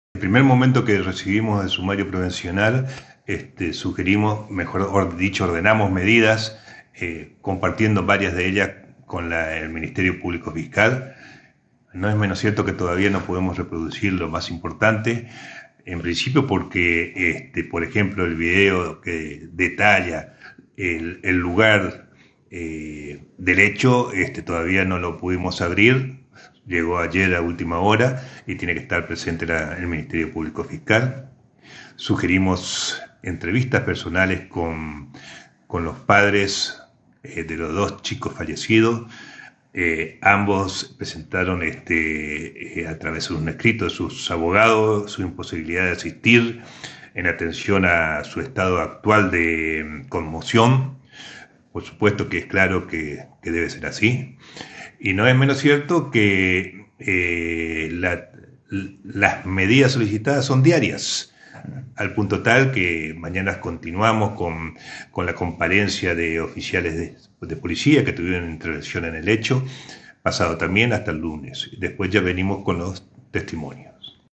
Posadas | Despiste fatal en la Costanera: aseguran que las cámaras de seguridad arrojarán certezas
🚨 En las últimas horas, la Policía de Misiones entregó al Juzgado Correccional y de Menores 2 la recopilación de las imágenes de las cámaras de seguridad desde el momento en que los menores salieron del establecimiento educativo hasta el momento del siniestro ocurrido el 11 de abril en la costanera de Posadas.

👉 Por otra parte, un informe de la Junta Médica determinó que los menores ya no presentan signos de estrés postraumático ni shock emocional tras el hecho.

🗣 En diálogo con Misiones OnLine, el juez César Jiménez, titular del Juzgado Correccional y de Menores 2, explicó: “Desde el primer momento de que recibimos el sumario policial hemos ordenado medidas, compartiendo varias de ellas con el Ministerio Público Fiscal. No es menos cierto que todavía no podemos reproducir lo más importante ya que el video que detalla el lugar del hecho este todavía no lo pudimos abrir. Llegó ayer a última hora y tiene que estar presente el Ministerio Público Fiscal”.

⭕️ Jiménez agregó: “Sugerimos entrevistas personales con los padres de los dos chicos fallecidos. Ambos presentaron a través de un escrito de sus abogados, su imposibilidad de asistir en atención a su estado actual de conmoción. Y no es menos cierto que las medidas solicitadas son diarias, al punto tal que mañana continuamos con la comparecencia de oficiales de policía que tienen intervención en el hecho. Después ya venimos con los testimonios”, contó el Magistrado.

❗️ Ante la consulta sobre el avance de la investigación, el juez respondió: “diariamente tenemos medidas que realizar que son las jornadas suscritas y sugeridas por Fiscal”.